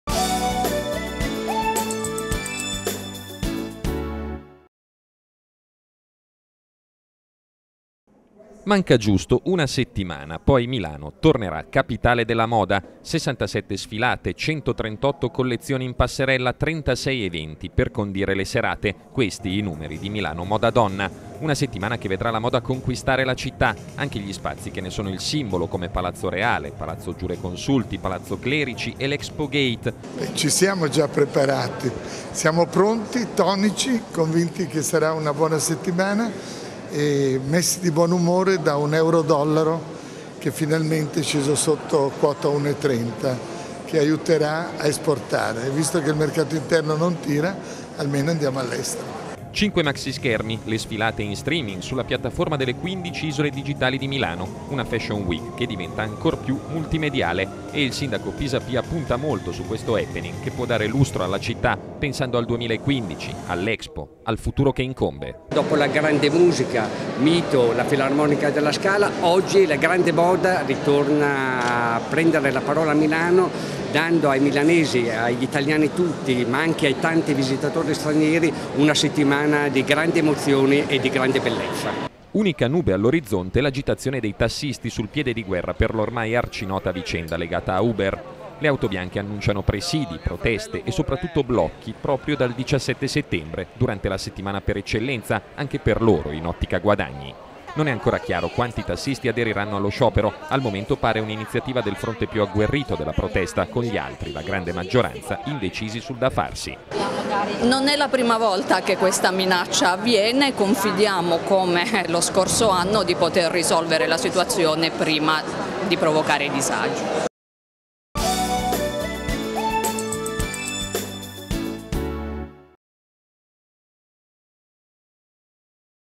Il 17/9 parte la Settimana della Moda, incubo taxi - TGN 10/09/14
Sarà un'edizione ricca, promettono Boselli e Pisapia. Sfilate anche in streaming, ma c'è l'incognita legata all'annunciato sciopero delle auto bianche